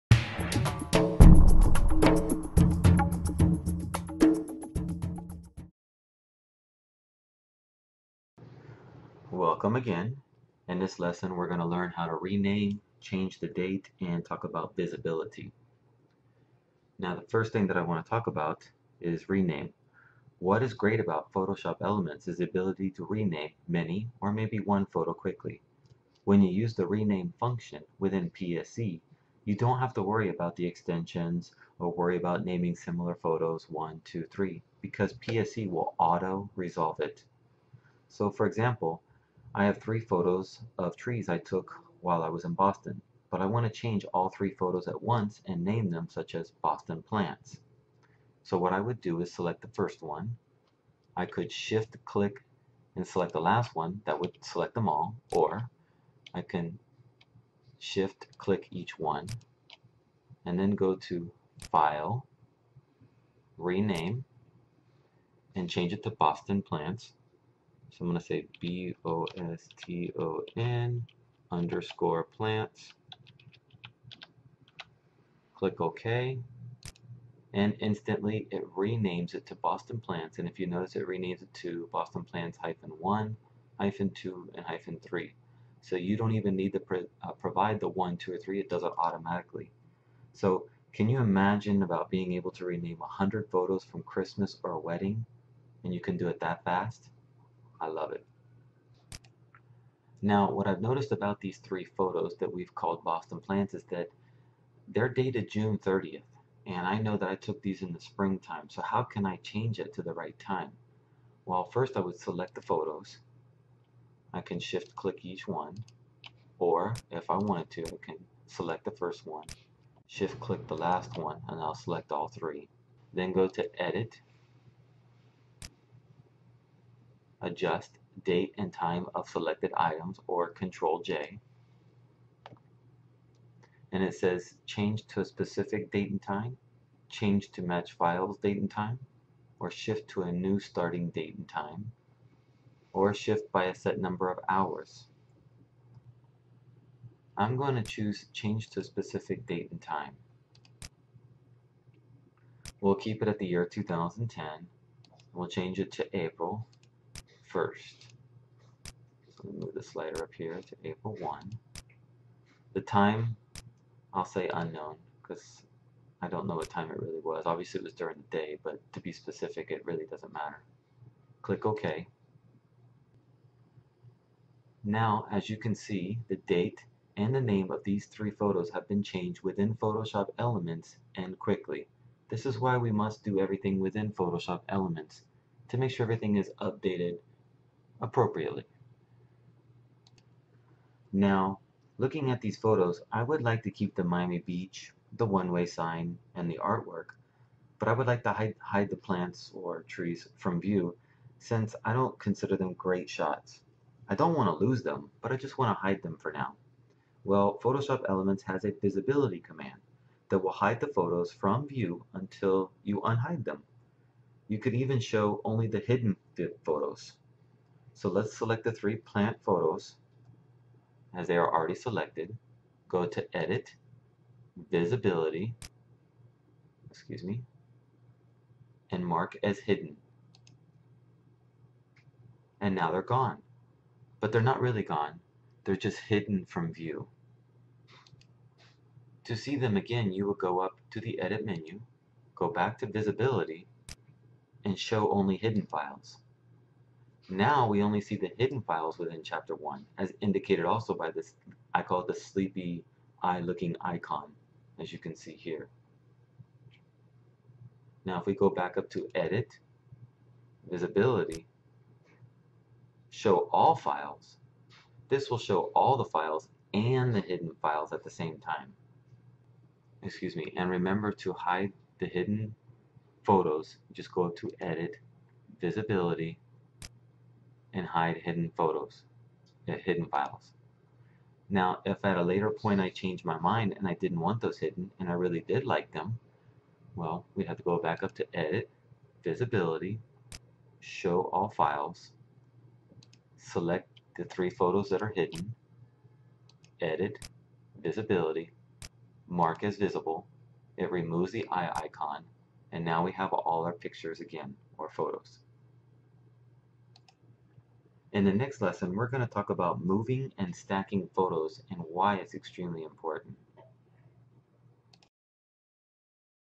Rename, Adjust Date/Time, and Visibility in Adobe Photoshop Elements 10
Learn how to rename, adjust date and time, or change visibility of files within Adobe Photoshop Elements 10.